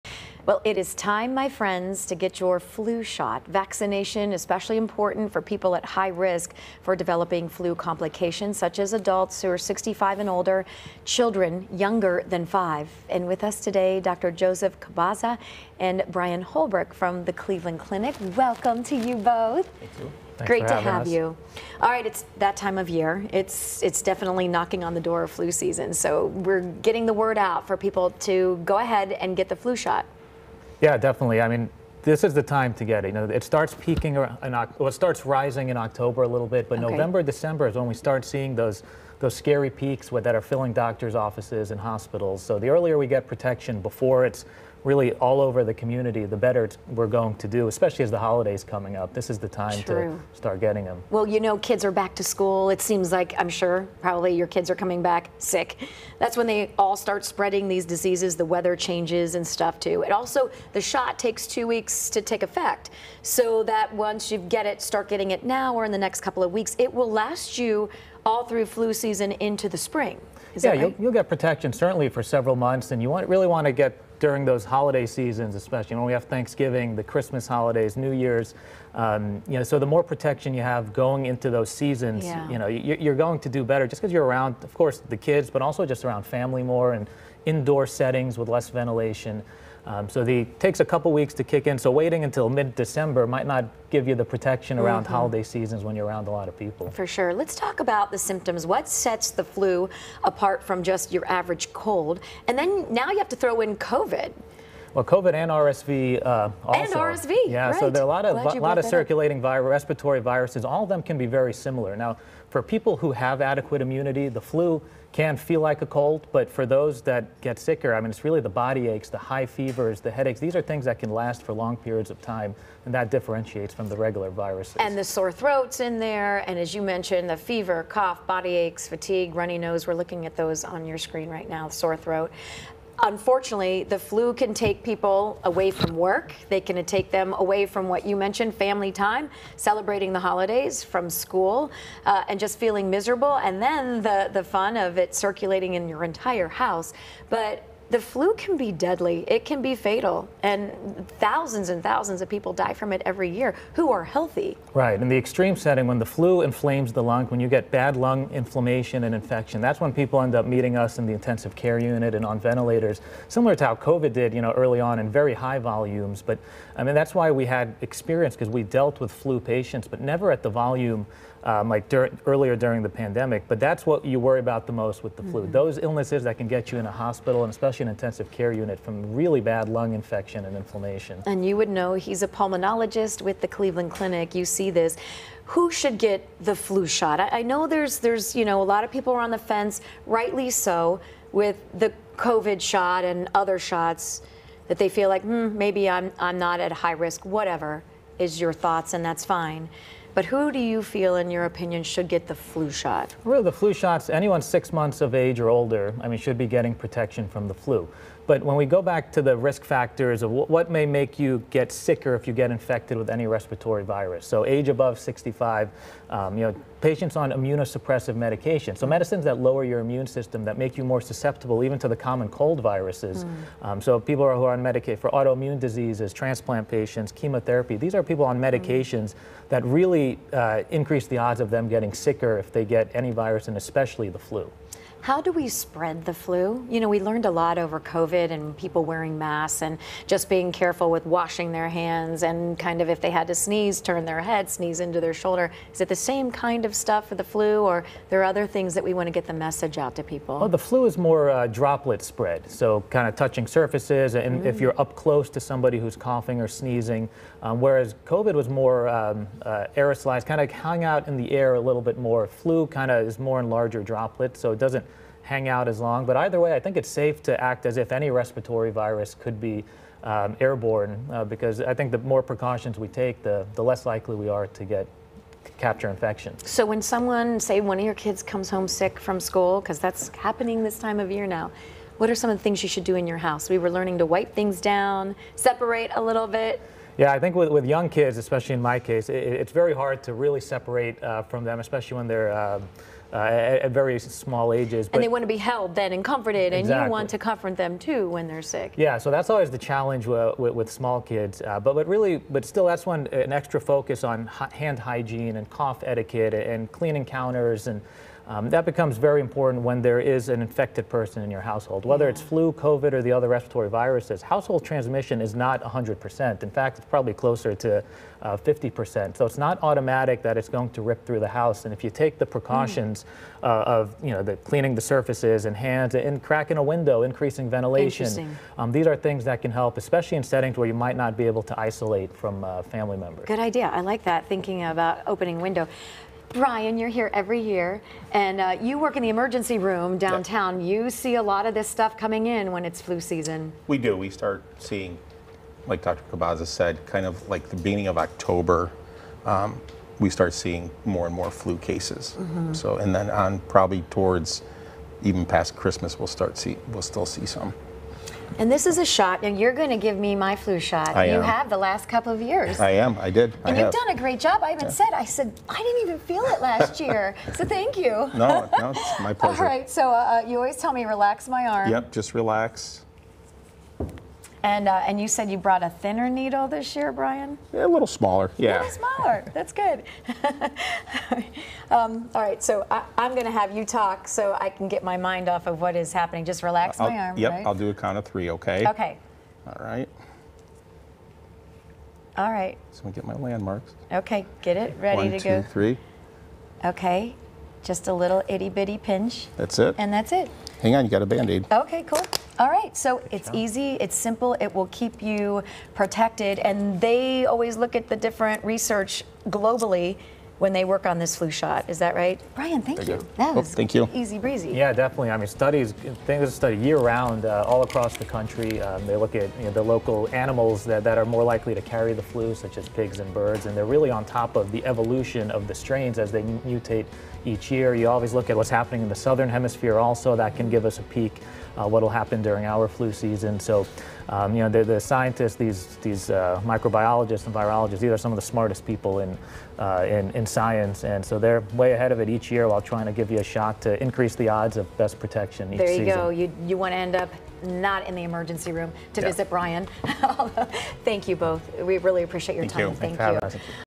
Stefani gets her flu shot and learns what's predicted for upcoming flu season
The Cleveland Clinic has begun it's awareness campaign to get people vaccinated for this year's flu season. Pulmonologist Dr Joseph Khabbaza explains why it's so important to be protected. Fox 8's Stefani Schaefer also learns what health experts are predicting for this year's flu season while she gets her flu vaccination.